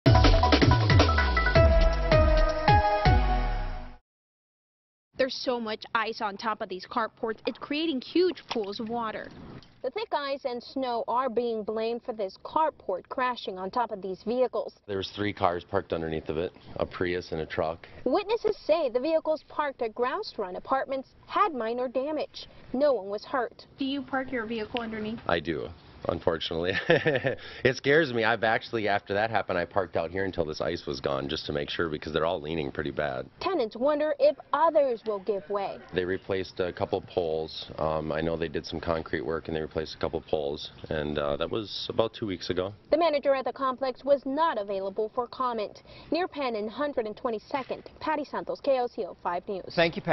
Carport crashes onto vehicles at OKC apartment complex after winter storm
The thick ice and snow is causing a lot of problems for people out on the road and parked outside their homes. Several vehicles parked under a carport at a Northwest Oklahoma City apartment complex were damaged when the metal building collapsed.